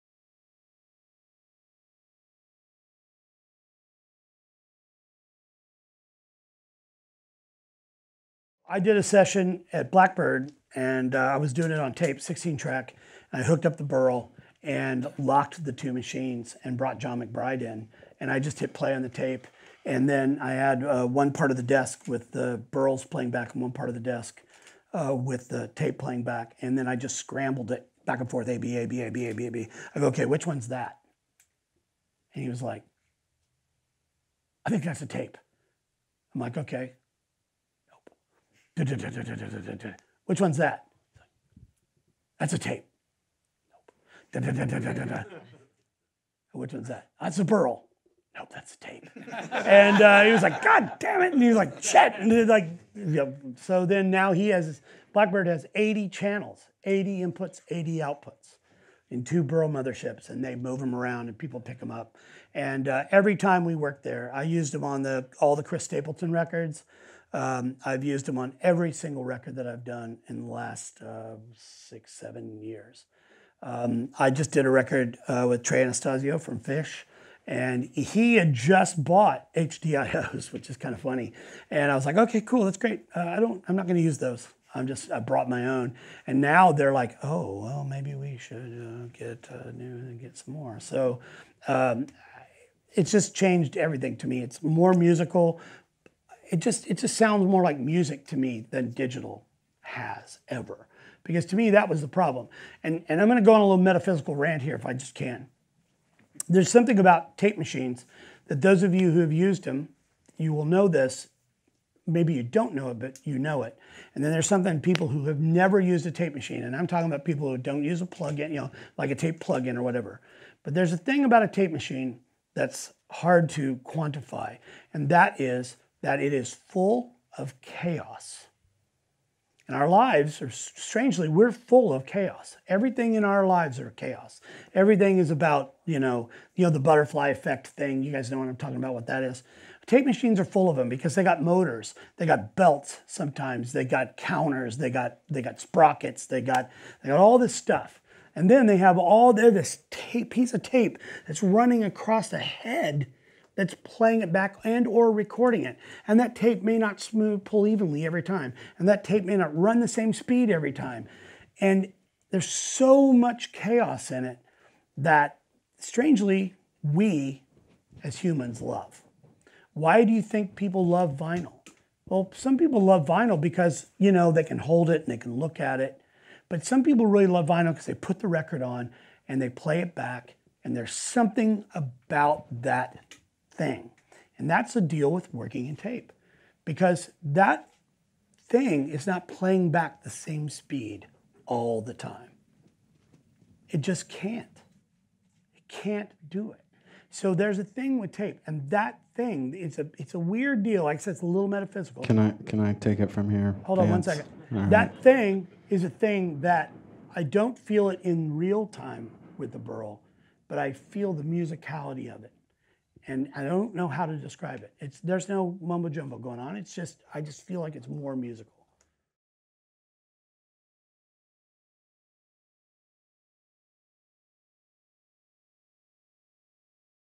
GRAMMY Winner Vance Powell Describes the Chaotic Musicality of Analog Tape
GRAMMY Award-winning recording engineer Vance Powell explains how the chaos of analog electronics creates the musical sound of analog tape and vinyl records. He found that the Burl Audio Mothership can capture that magic when recording from analog to digital. From Jack White, Chris Stapleton, and The Revivalists, Vance Powell has used his Burl Audio system on every record for the past 6+ years. He even convinced John McBride, owner of Blackbird Studios in Nashville to get a complete Burl Audio system after a blind A/B test!

Click the link to check out proSESSIONS at Westlake Pro for more exclusive interviews with industry leaders.